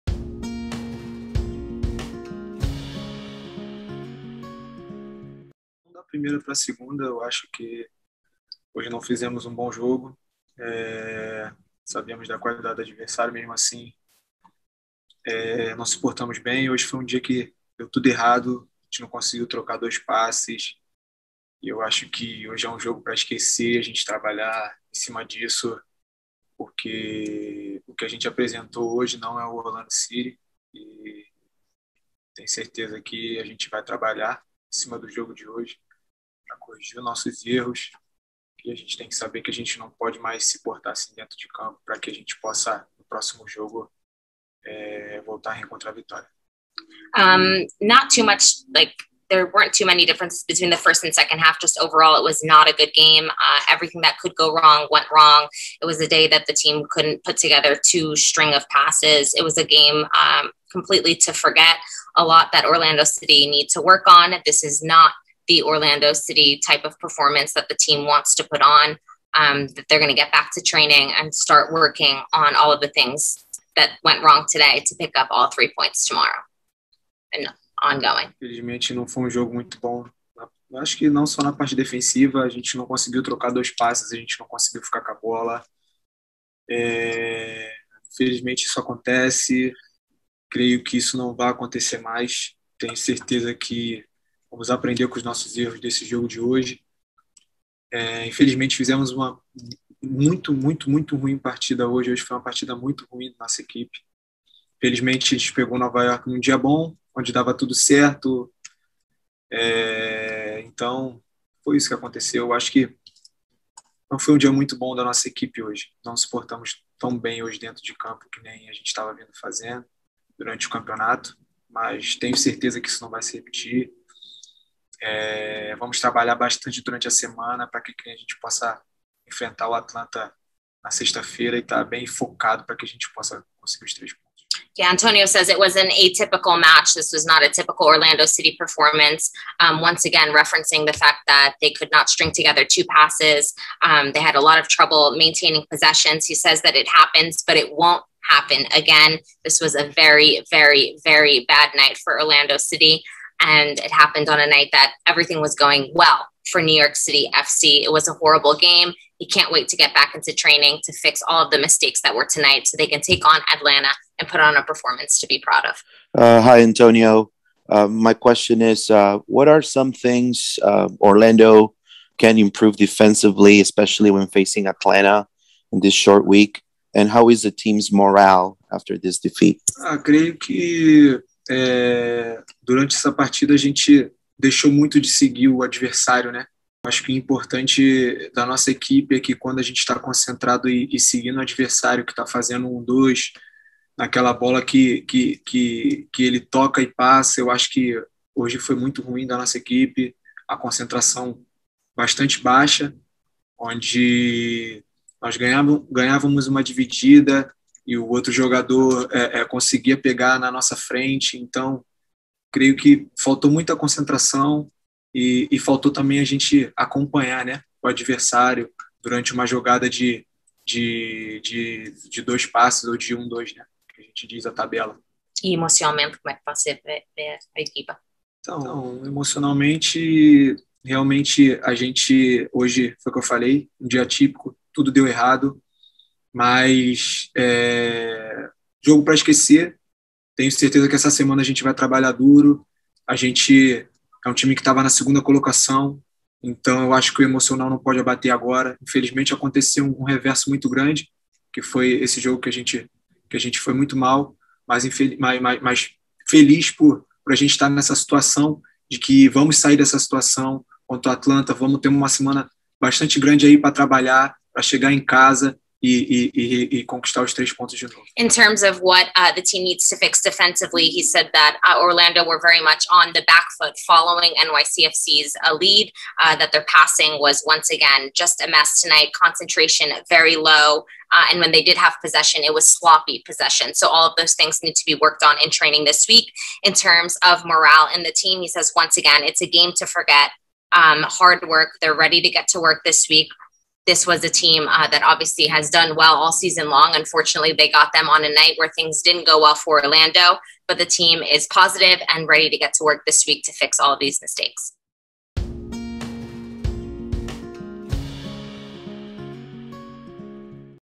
Antonio Carlos | Orlando City | Postgame press conference
🗣 Antonio Carlos 🇧🇷

NYCFC vs. Orlando City 07.25.21

🎥 Credit: Orlando City Communications
📸 Orlando City Communications